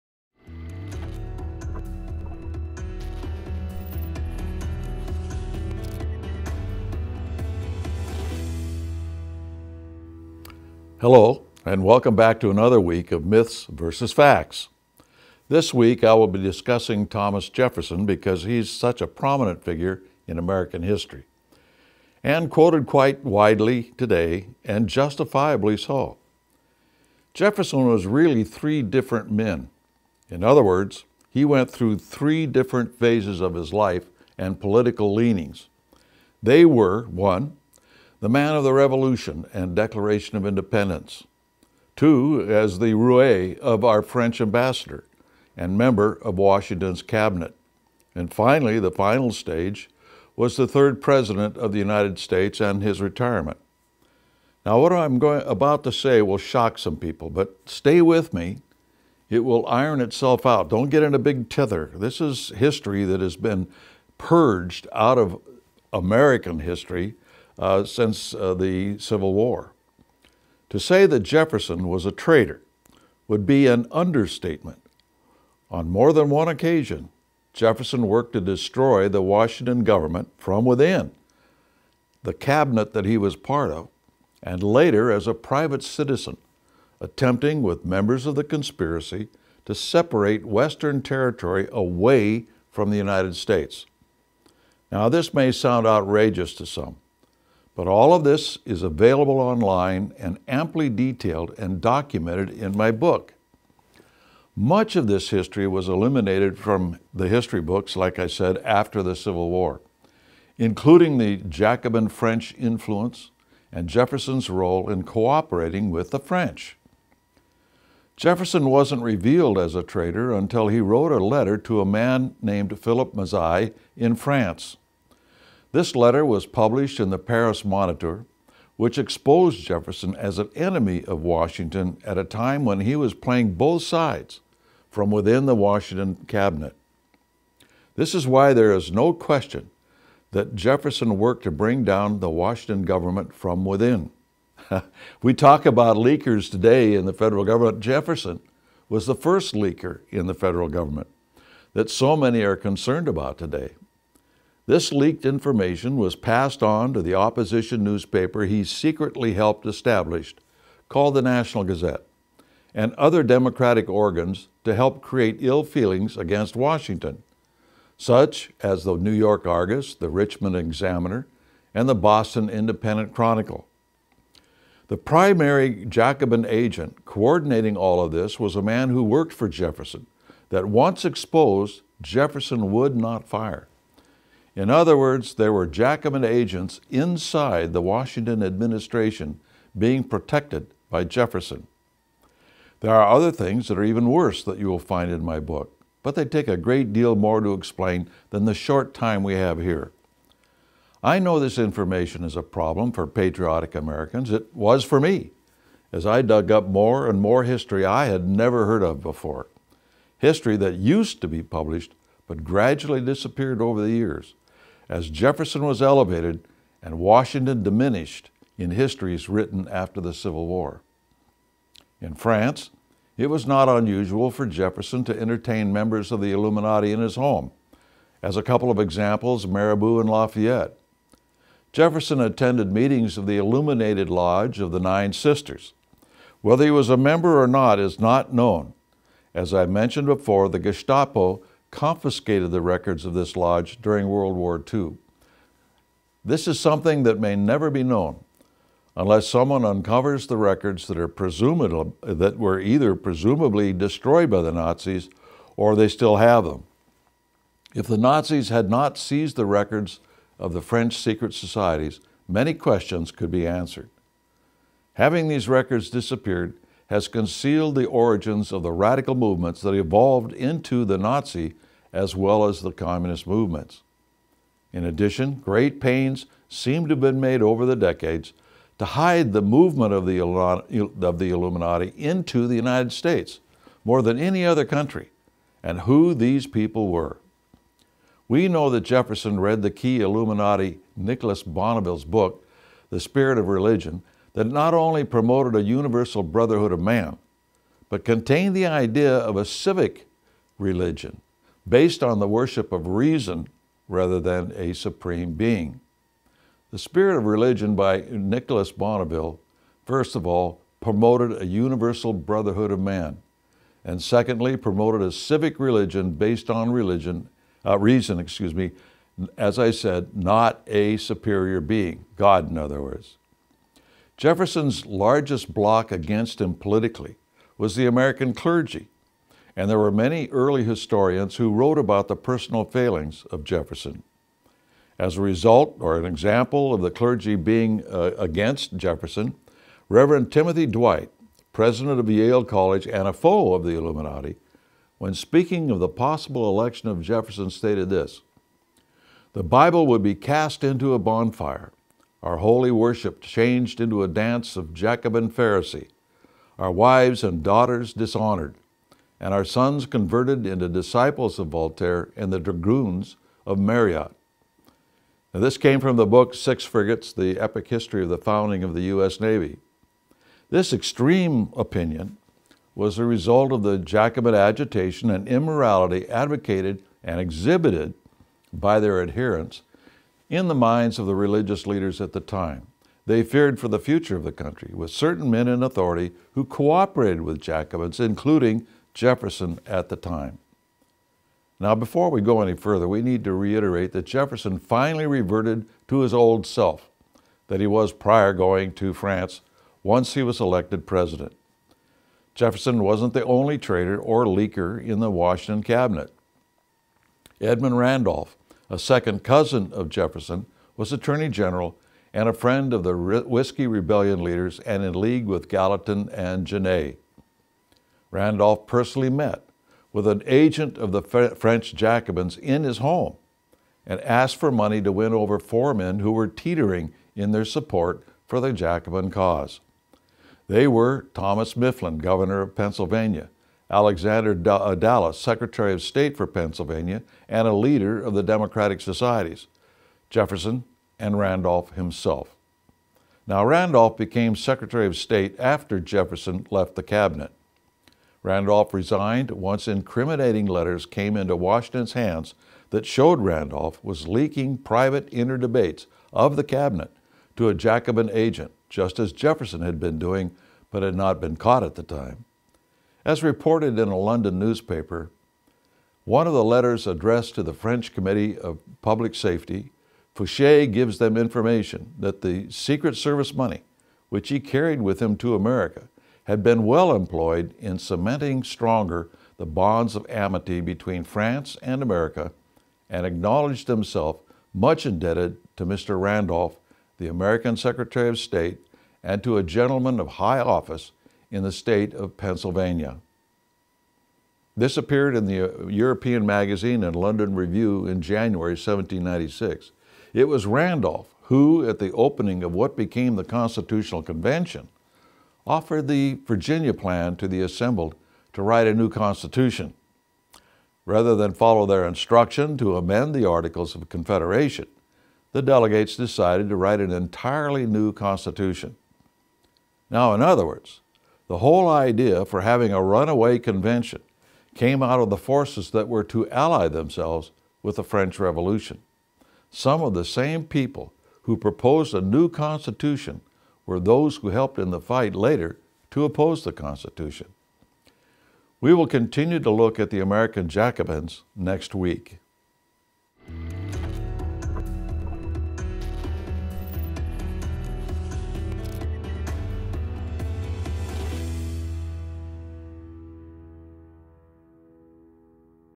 Thomas Jefferson | What You Didn’t Know
In episode 13 of Myths VS Facts Art Thompson uncovers details from Thomas Jefferson's life that you wont learn in history books today.

LIKE THE JOHN BIRCH SOCIETY AND WANT TO GET INVOLVED?